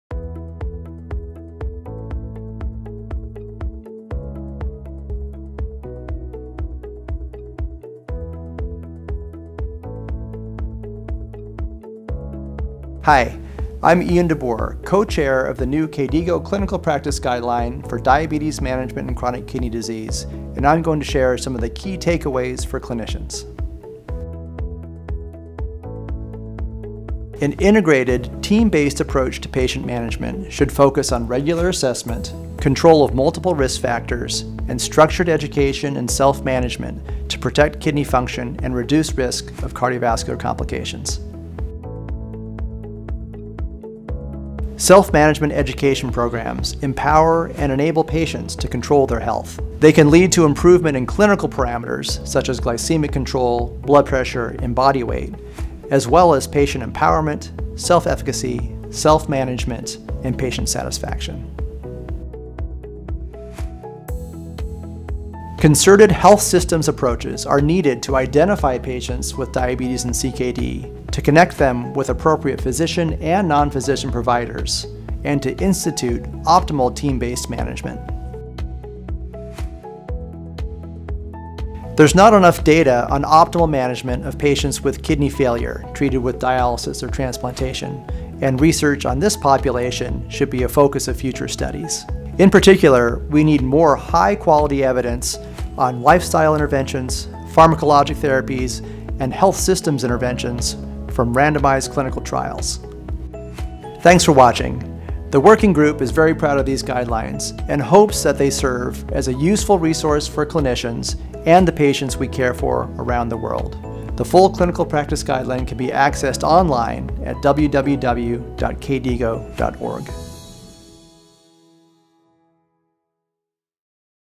KDIGO Top 10 Takeaways for Clinicians from the 2020 Diabetes Management Guideline: Module 3
Dr. Ian de Boer, Co-Chair of the 2020 KDIGO Diabetes Management in CKD Guideline, walks you through the Top 10 Takeaways for Clinicians. Module 3 explores optimal management.